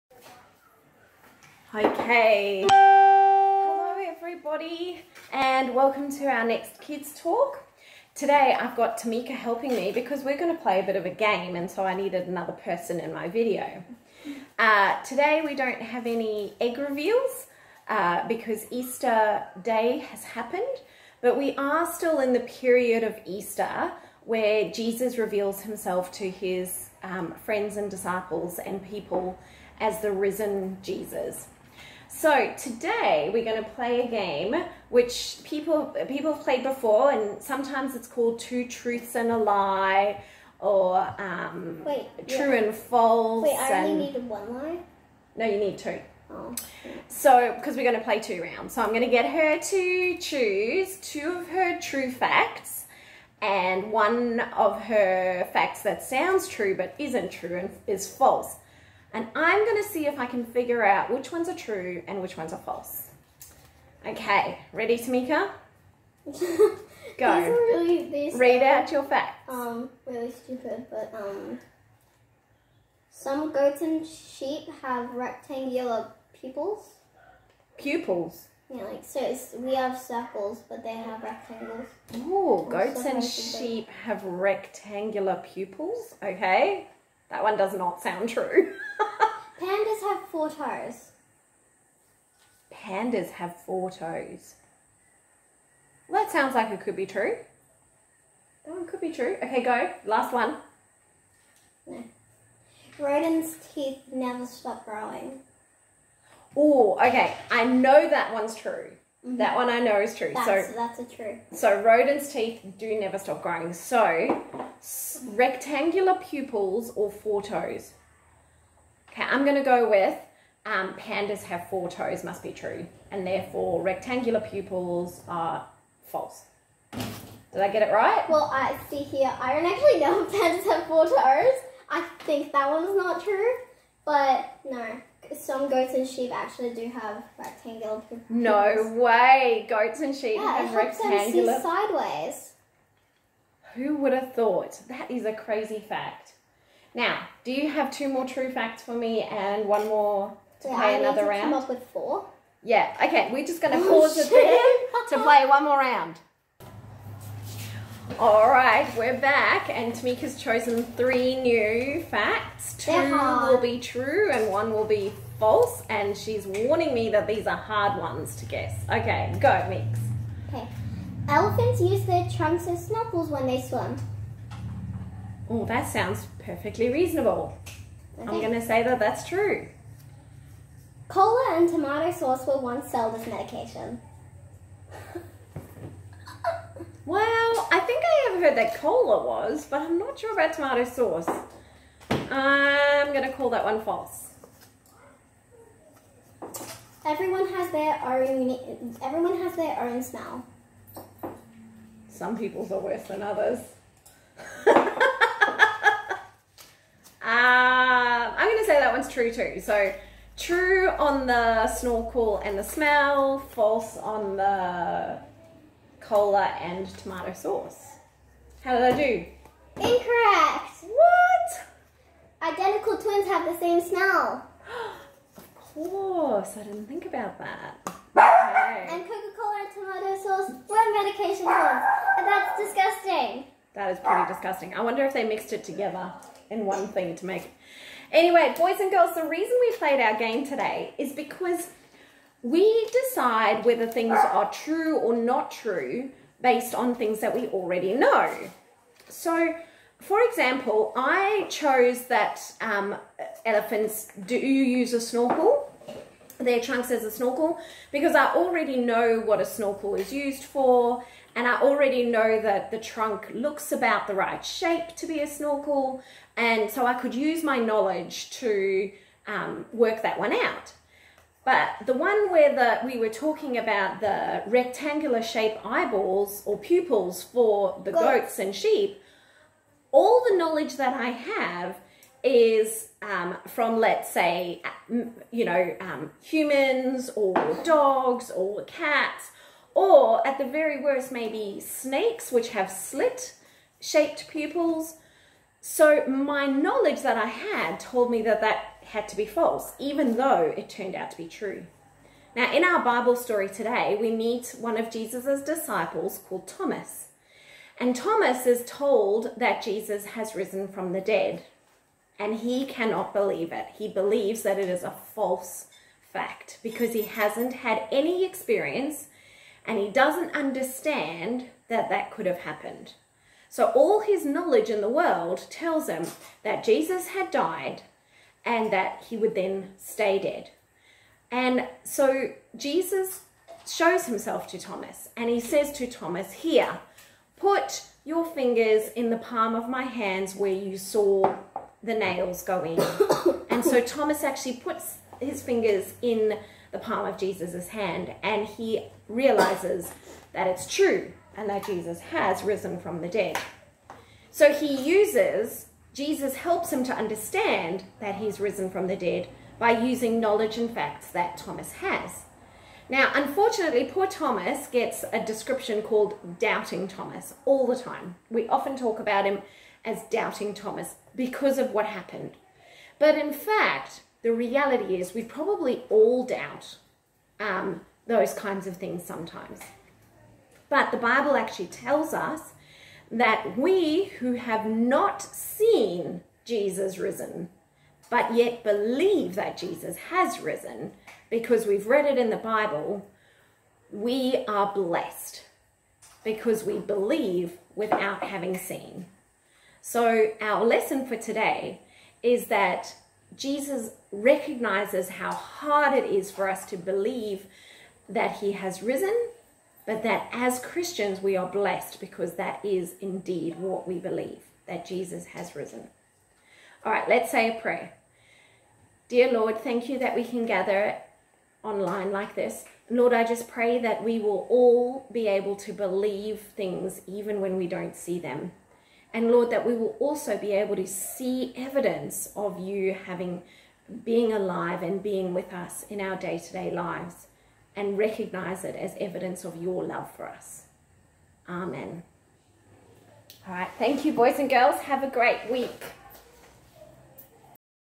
Kids Church - Doubting Thomas
Tamika and Lauren play a game to demonstrate just how difficult it can be to believe the unbelievable.